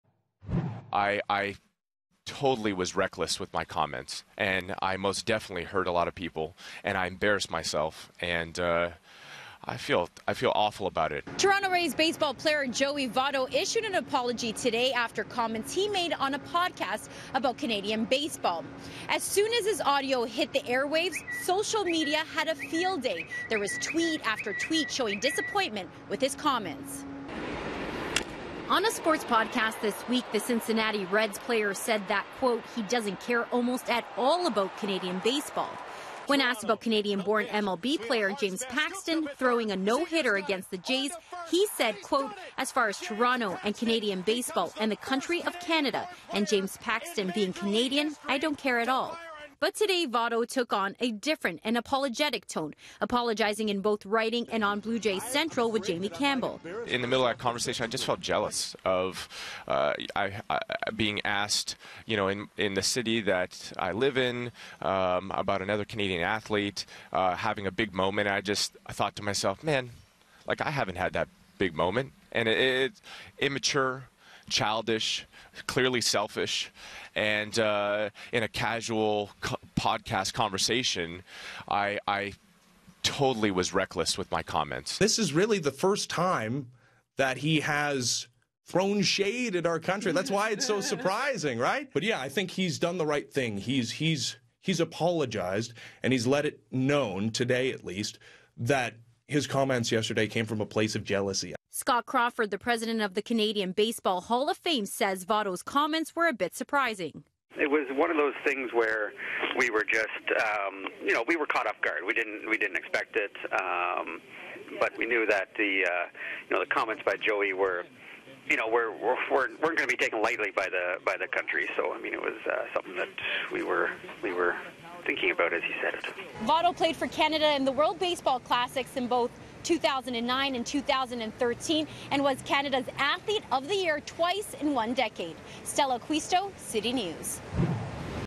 Votto apologizes for remarks ripping Canadian baseball
Saying his comments came from an ``absolutely silly, short-sighted, selfish place,'' Cincinnati Reds slugger Joey Votto apologized Wednesday for saying that he doesn't care about Canada, Canadian baseball or his hometown of Toronto.